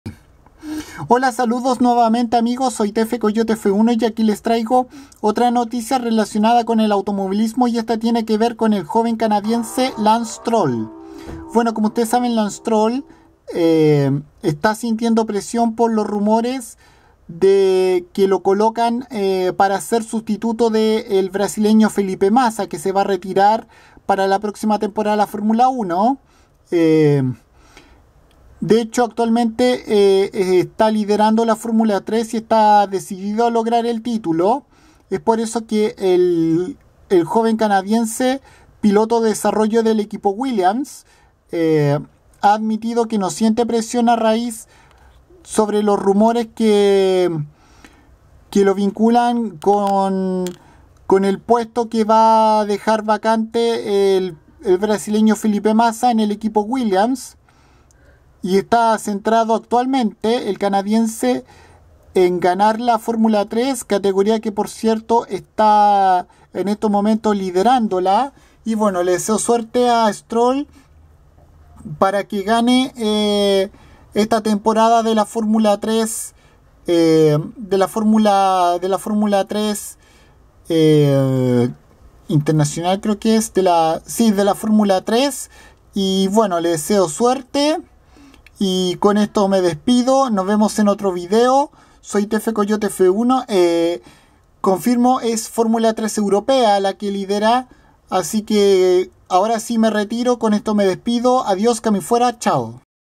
Lance Stroll suena como rumor para sustituir a Felipe Massa en Williams para 2017
El joven piloto canadiense actual líder de la F3 europea,Lance Stroll,suena como uno de los rumores en el equipo Williams para ser el sustituto del brasileño Felipe Massa,quien dejará la F1 a fines de la actual temporada 2016 mientras,el joven canadiense piensa en ganar el título de campeón de su actual categoría